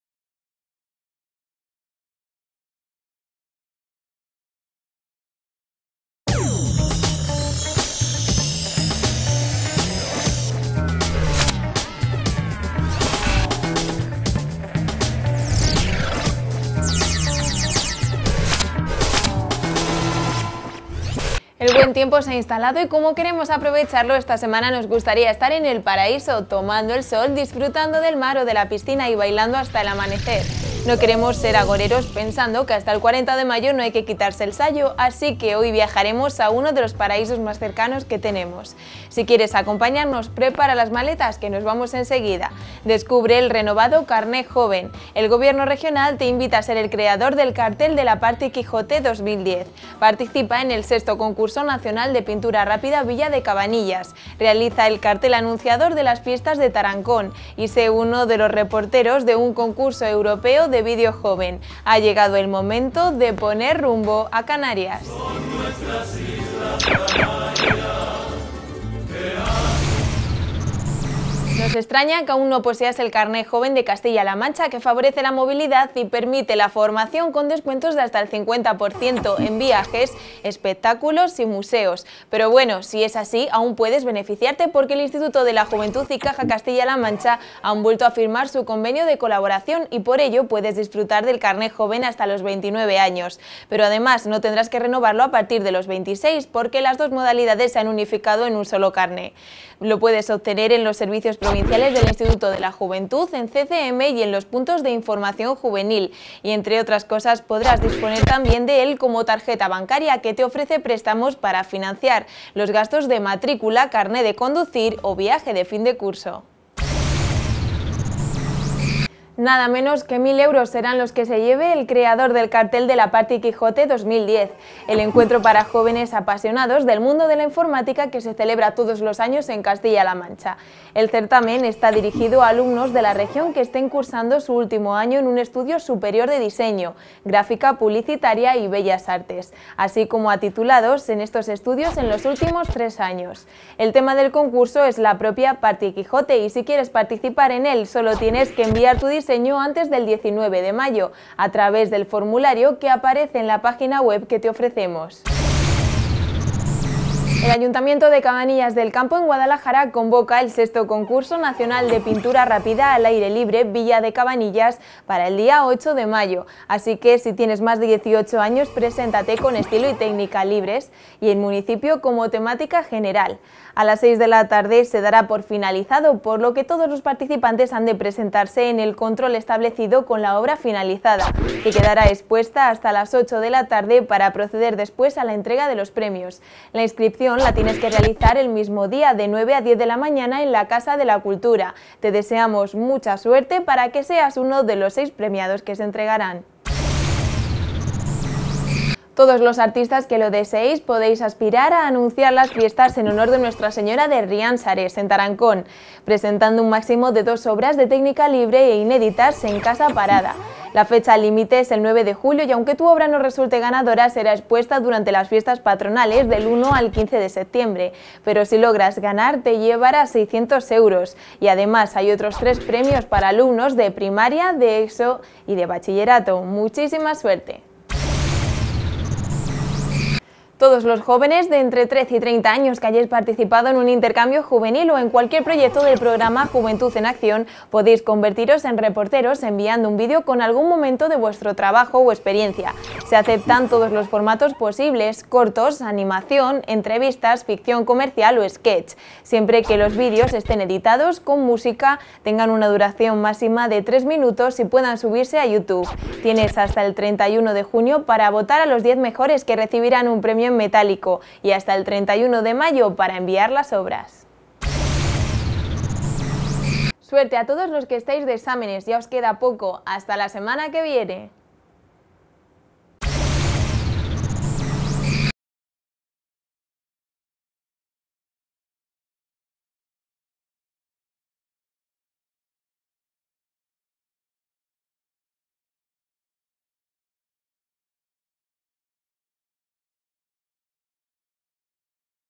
Juventud Informa número 144
El buen tiempo se ha instalado y como queremos aprovecharlo esta semana nos gustaría estar en el paraíso tomando el sol, disfrutando del mar o de la piscina, y bailando hasta el amanecer. No queremos ser agoreros pensando que hasta el 40 de mayo no hay que quitarse el sayo, asique hoy viajaremos a uno de los paraísos más cercanos que tenemos, si quieres acompañarnos prepara las maletas, que nos vamos enseguida: descubre el renovado Carné Joven; el Gobierno Regional te invita a ser el creador del Cartel de la Party Quijote 2010; participa en el 6º Concurso Nacional de Pintura Rápida Villa de Cabanillas; realiza el Cartel Anunciador de las Fiestas de Tarancón y se uno de los reporteros de un concurso europeo de Vídeo Joven. Ha llegado el momento de poner rumbo a Canarias!!!